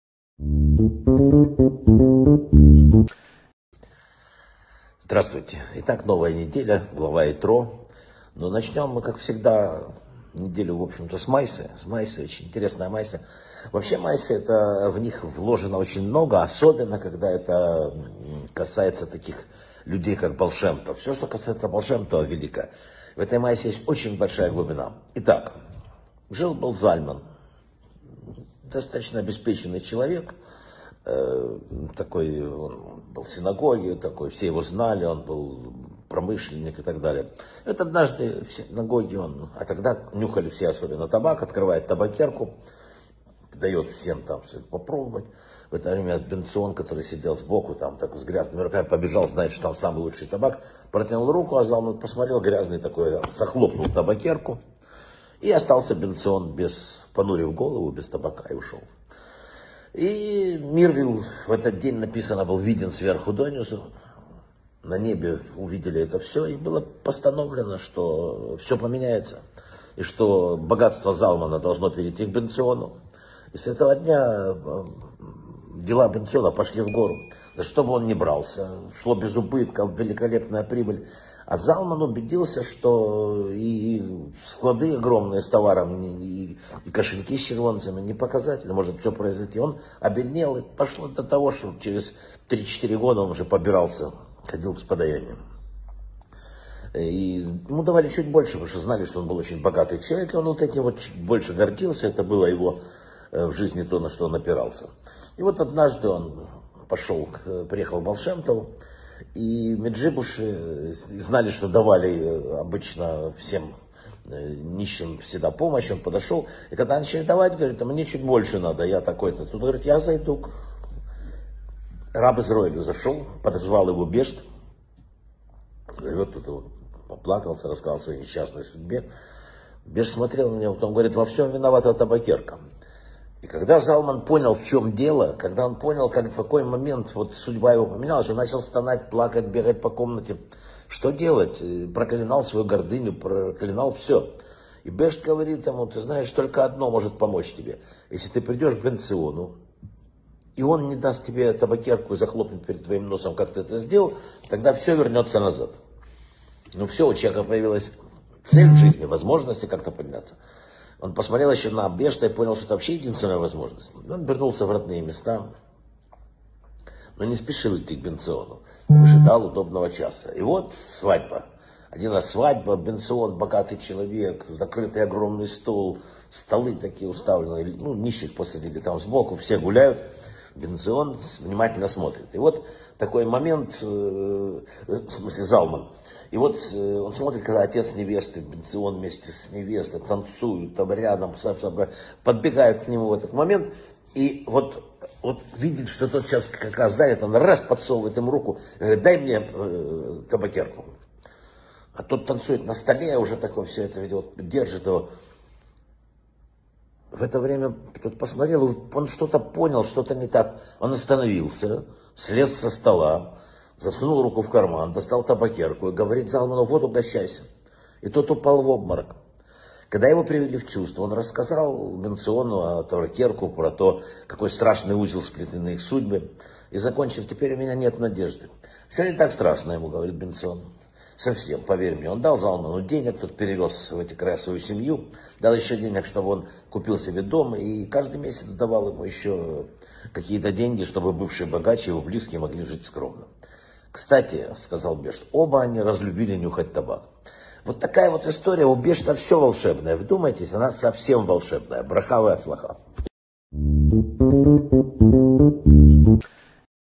📘 Гордыня богача и мудрость Баал Шем Това. Майса о табакерке | Вадим Рабинович
Мудрость праведников раскрывается в историях о них - сегодня слушаем историю о двух евреях, один из которых потерял свое богатство и отправился к Баал Шем Тову.
В те времена многие любили нюхать табак, и промышленник Залман угощал присутствующих. Кому он не дал табака и почему?
Что увидели на Небесах в этот день и как изменилась жизнь Залмана?
На что продолжал опираться этот человек в жизни, даже потеряв все? Какой совет дал ему Баал Шем Тов?
В какой момент Залман решил прийти к Бенциону, чтобы все исправить, и что вышло в итоге?
Как Бенцион проявил милосердие и мудрость, узнав о целях Залмана? Урок о том, как доброта и праведность в отношениях с ближними преодолевает все проблемы и дает пример достойного поведения.